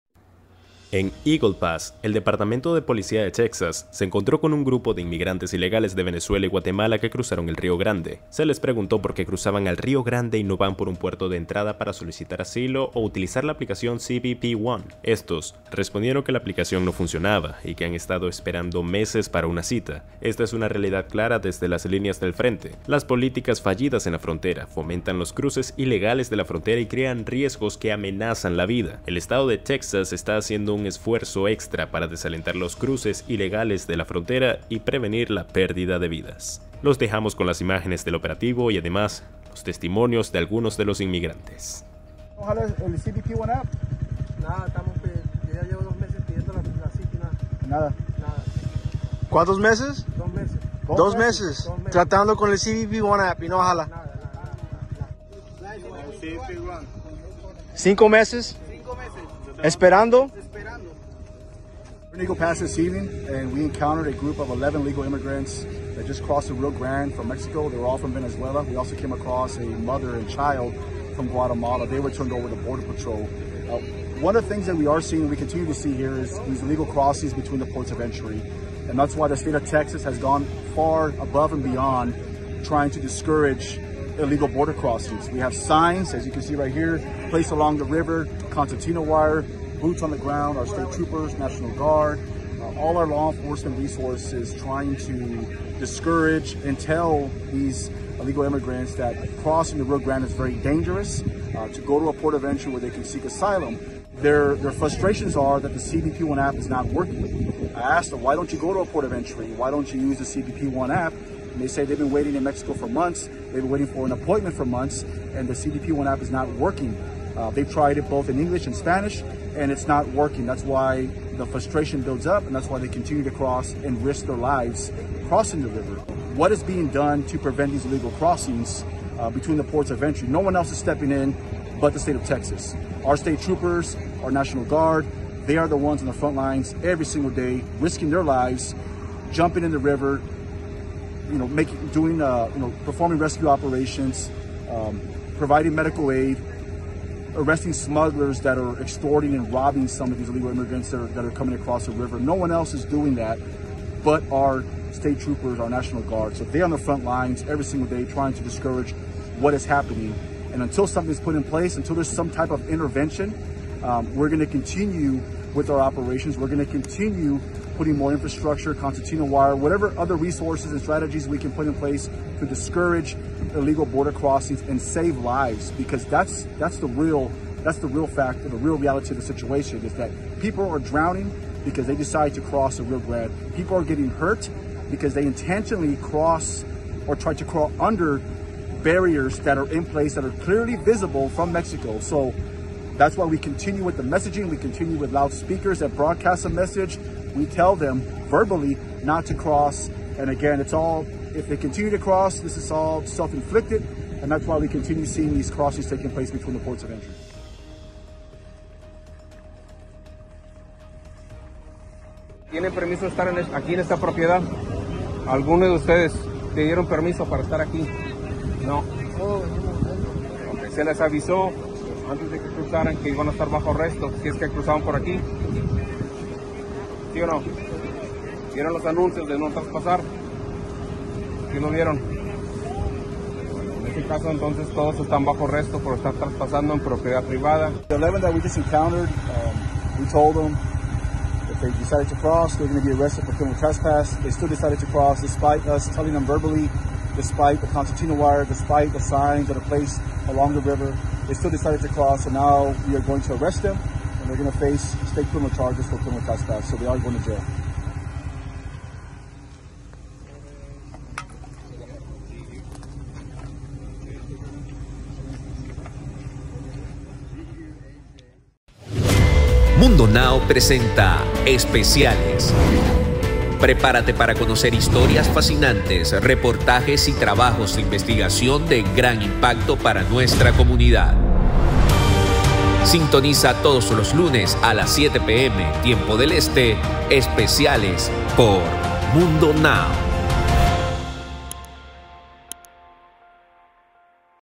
Crisis en la Frontera: Historias de Inmigrantes Detenidos en Eagle Pass, Texas
Este video documenta las experiencias de inmigrantes ilegales de Venezuela y Guatemala detenidos en Eagle Pass, Texas. Descubre las realidades difíciles y desgarradoras que enfrentan los inmigrantes mientras esperan durante meses para solicitar asilo. Aprende cómo las políticas fronterizas y la falta de acceso a los servicios necesarios están alimentando la crisis. Testimonios en vivo, reportajes de investigación y una visión de primera mano de las operaciones en la frontera. Sintoniza todos los lunes a las 7pm ET para más especiales de impacto en nuestra comunidad.